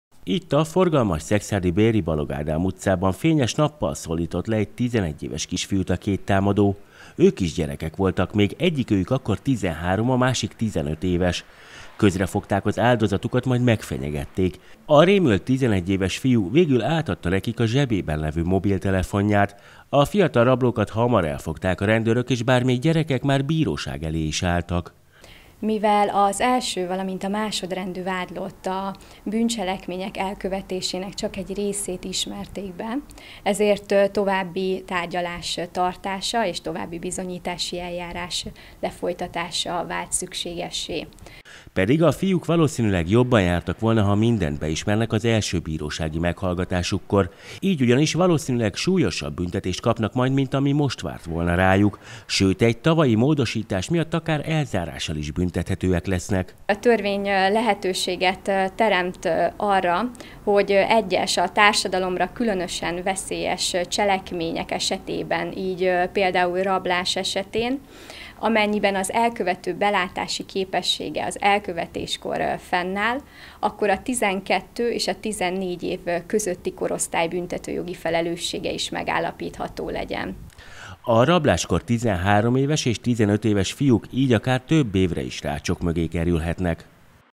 GYEREKEK RABOLTAK KI EGY KISFIÚT SZEKSZÁRDON - akár börtönt is kaphatnak tettükért
13 és 15 éves fiúk raboltak ki egy 11 éves áldozatot Szekszárdon fényes nappal. A telefonját vették el tőle, miután megfenyegették, hogy megverik. A rablók – bár még gyerekek – börtönbe kerülhetnek. Tavaly óta ugyanis súlyos bűncselekmények esetén már 12 éves kortól büntethetőek az elkövetők Magyarországon.